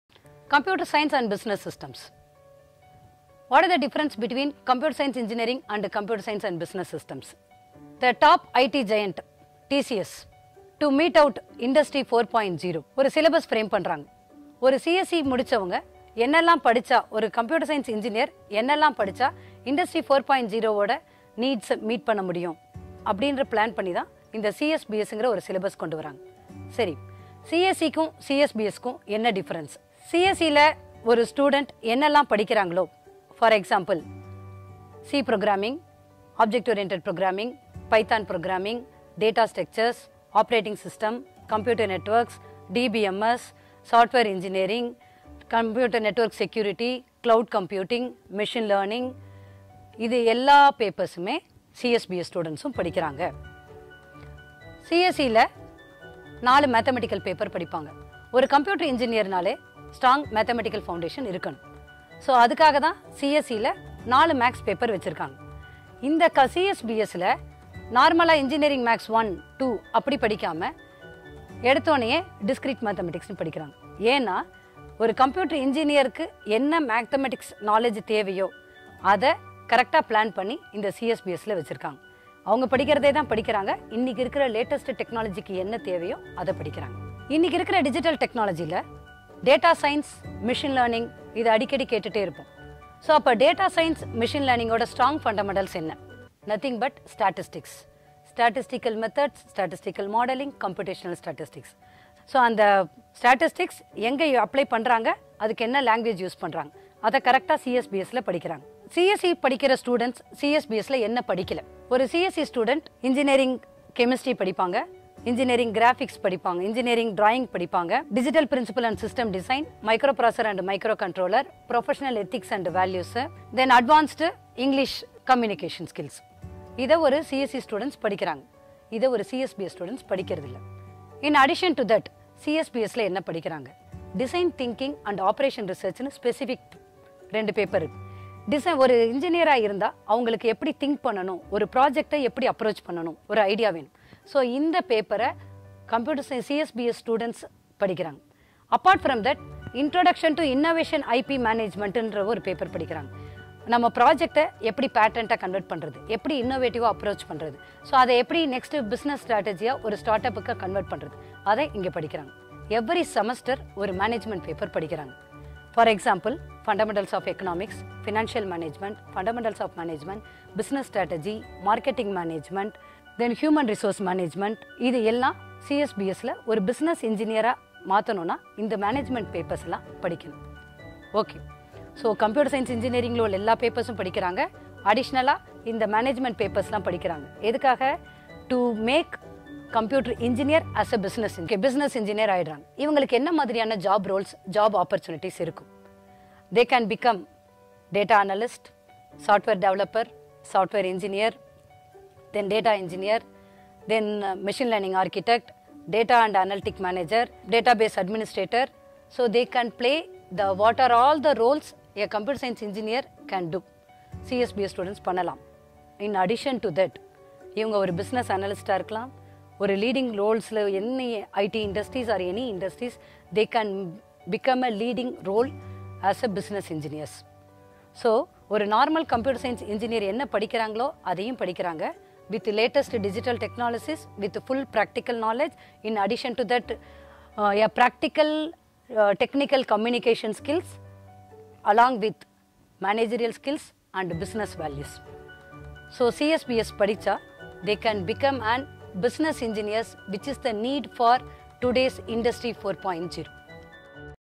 KNOW MORE ABOUT CSBSCOMPUTER SCIENCE AND BUSINESS SYSTEMS
educationalist  and Academician Dr.M.Ananthi / HOD /CSBS Sri Sairam Engineering College, Chennai. explain about course